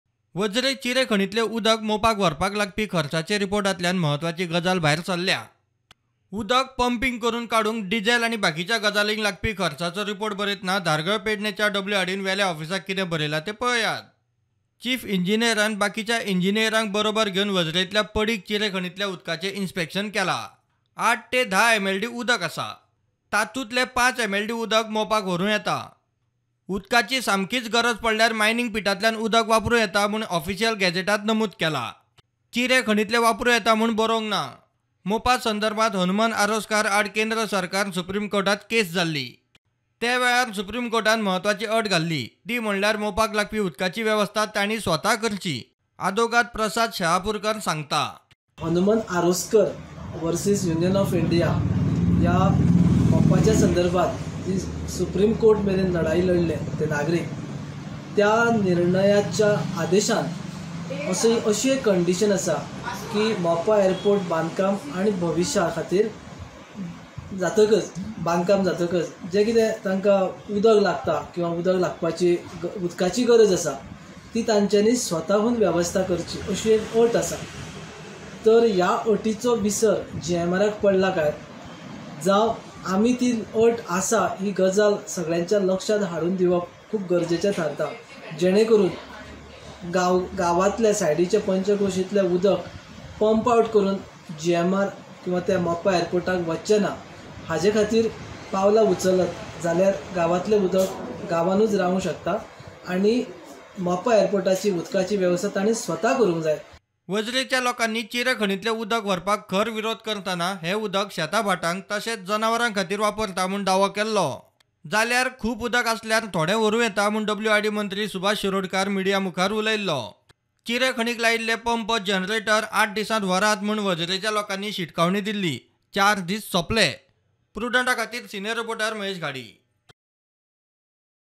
BIG REVELATION IN MOPA AIRPORT WATER FIASCO | Prudent Media Goa
New revelations have come to light in the case where the WRD has been pumping out water from the stone quarry to supply it to the Mopa International Airport.

There is around 8 to 10 MLD of water in the stone quarry, of which 5 MLD can be used for Mopa. Dhargal-Pernem WRD has stated that the Chief Engineer has conducted the inspection.

Activists Advocate Shahapurkar has said that the project owners themselves should provide water to Mopa, it is the order of the Supreme Court.